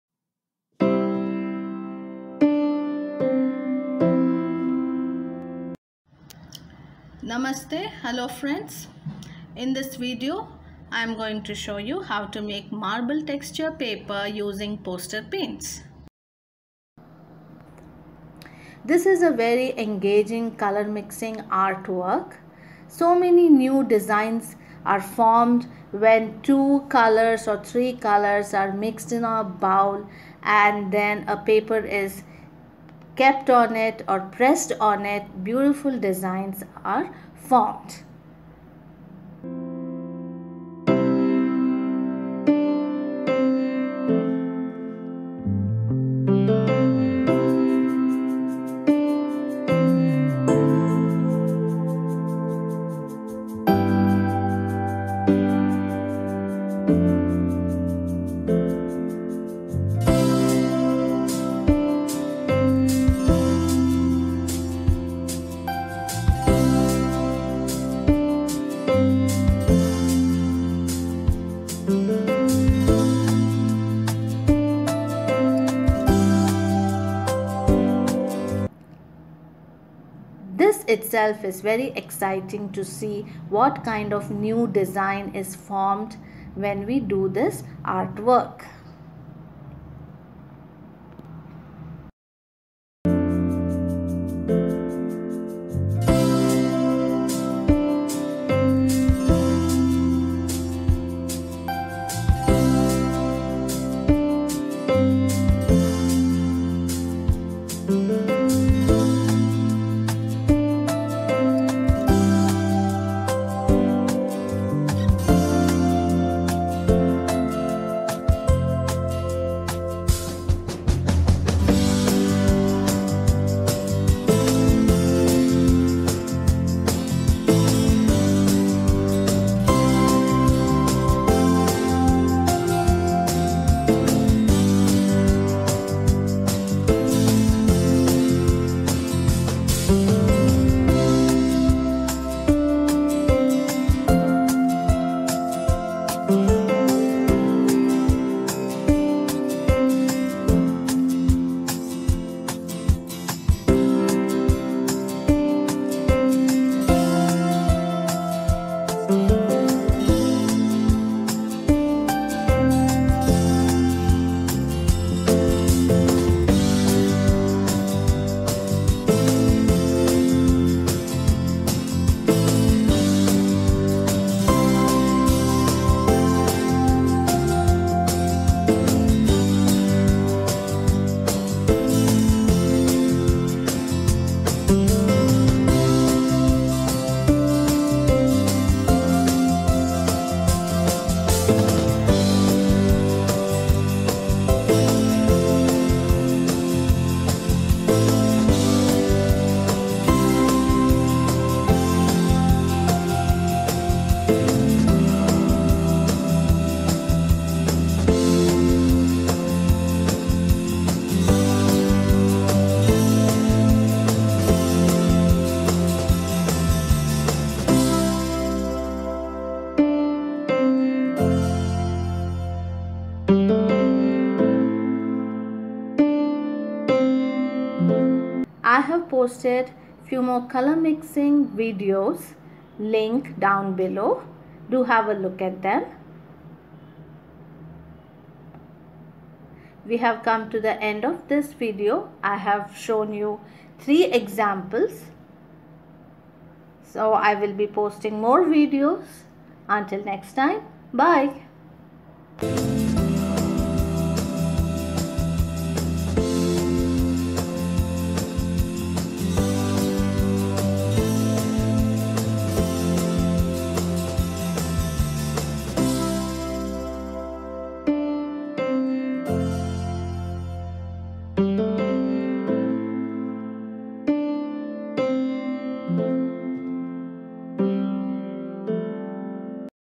How to create marble texture using Poster Paints.
How to make MARBLE Texture  on paper. Poster paints marbling technique. Marbling paper fun art activity. Colour mixing activity